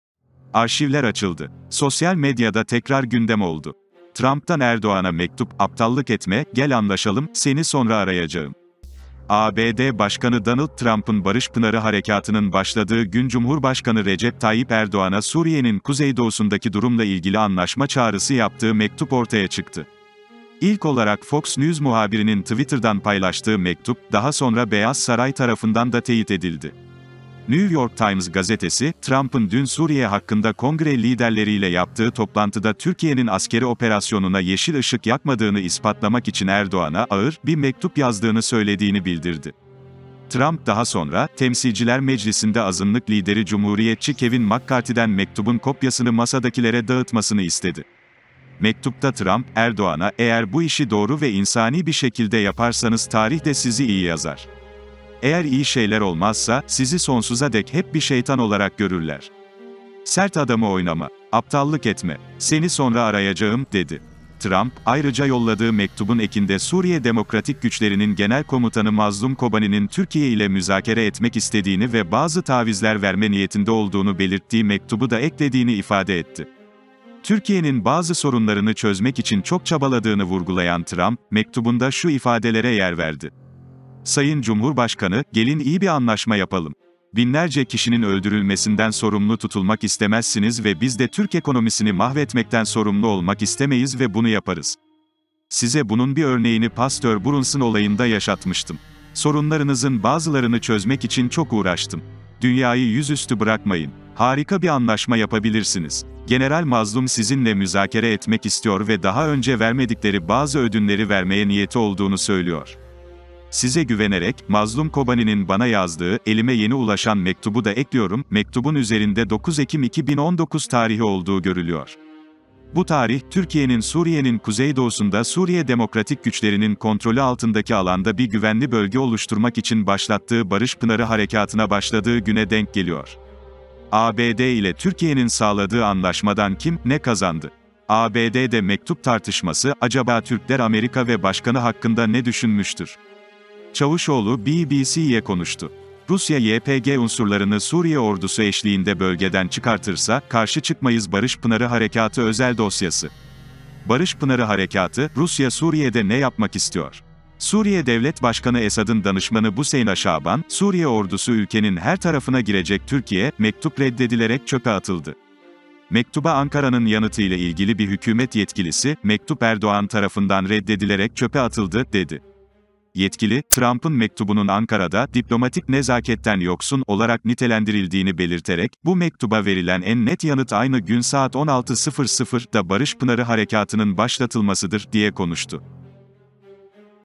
Erdoğan'ı Çileden Çıkaran Mektup İfşa Oldu: Aptallık etme, gel anlaşalım!
ABD Başkanı Donald Trump'ın Barış Pınarı Harekatı'nın başladığı gün Cumhurbaşkanı Recep Tayyip Erdoğan'a Suriye'nin kuzeydoğusundaki durumla ilgili anlaşma çağrısı yaptığı mektup ortaya çıktı.